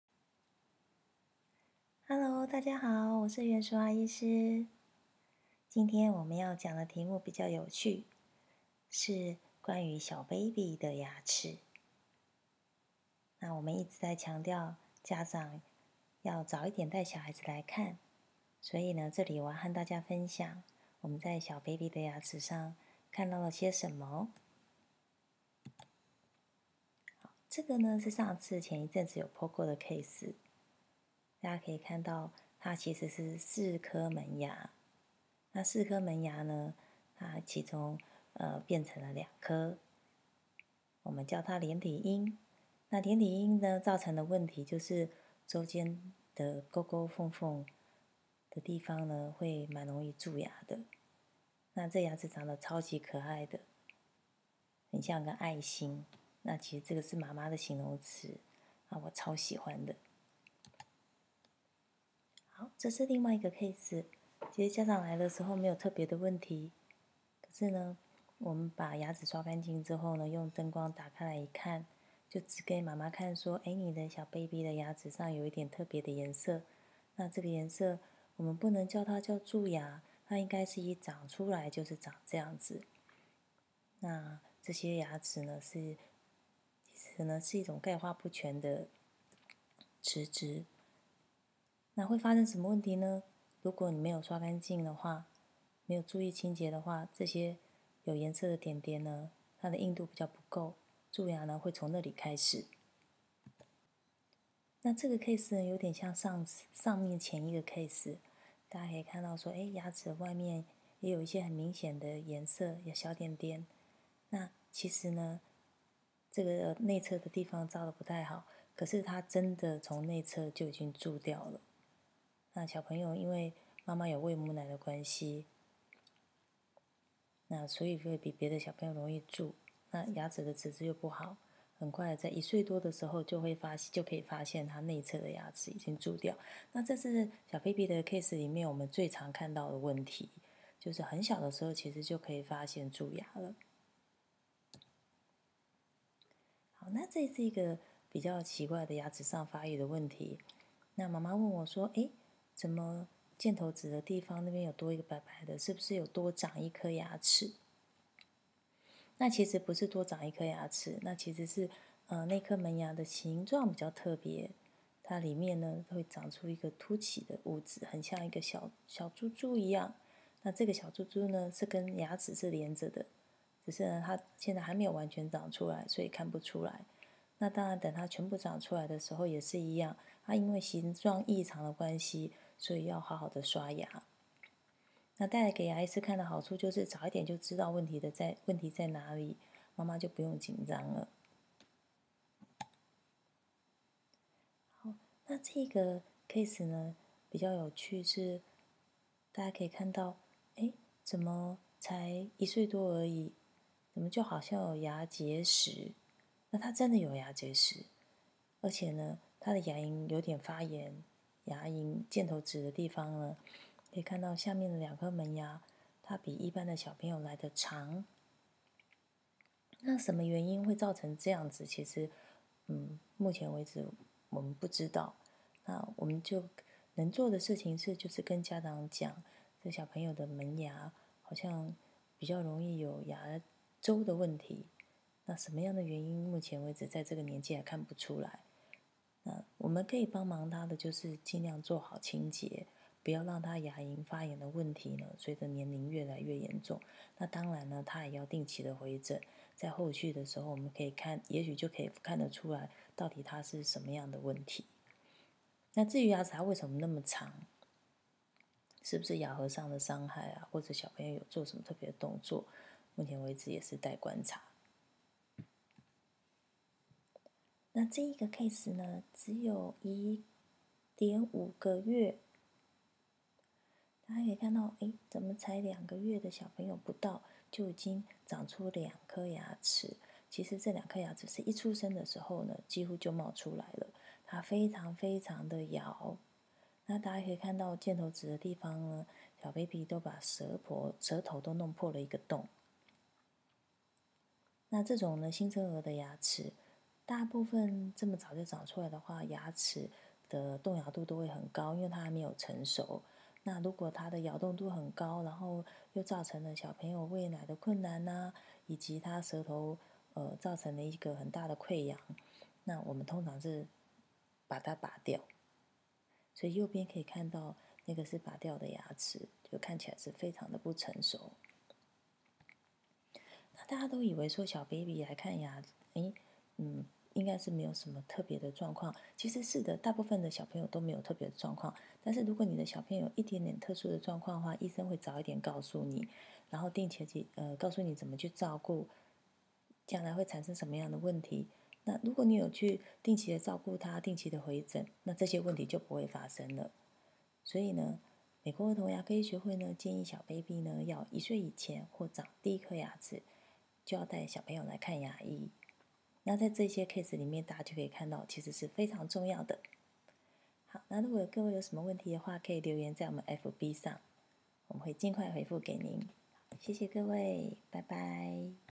小baby看牙醫; Oral findings for babies under than 2 years old
早ㄧ點帶baby來看牙，醫師可以幫忙發現許多先天性型態或齒質的異常，給予適當的照顧，未來就不會發生蛀牙或牙周的問題。
用這些病例和大家分享，小小孩看牙的必要性！

美國兒童牙醫學會建議一歲以前要帶孩子看牙醫！兒童牙醫；齒列矯正